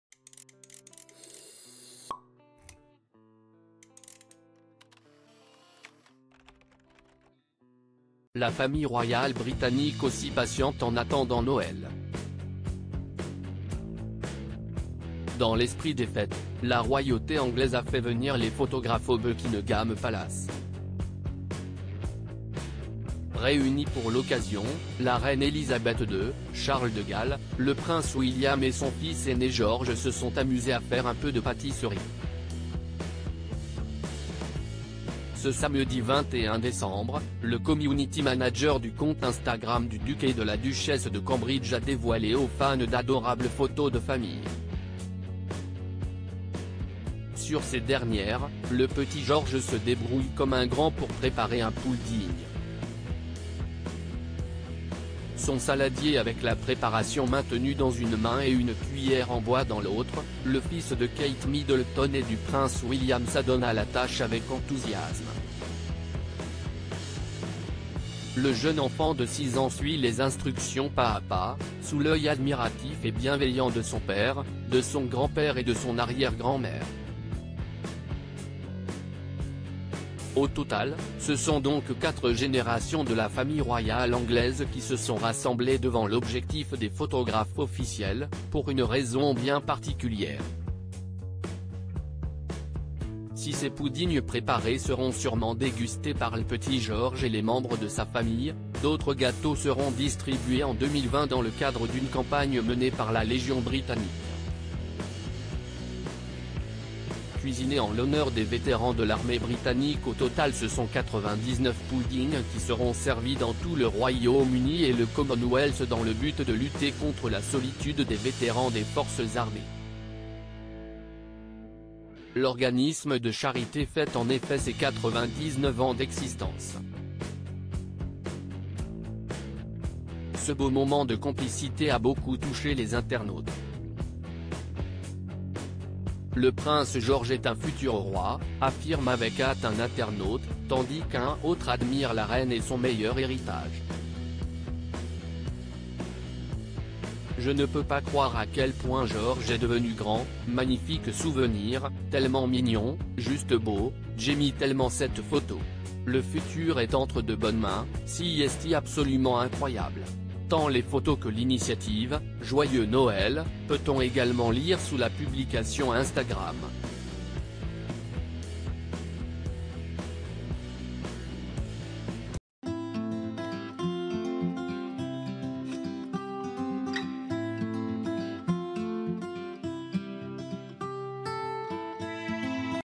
Quand le Prince George fait la cuisine avec son père, son grand-père et son arrière-grand-mère, la R
Quand le Prince George fait la cuisine avec son père, son grand-père et son arrière-grand-mère, la Reine (photos)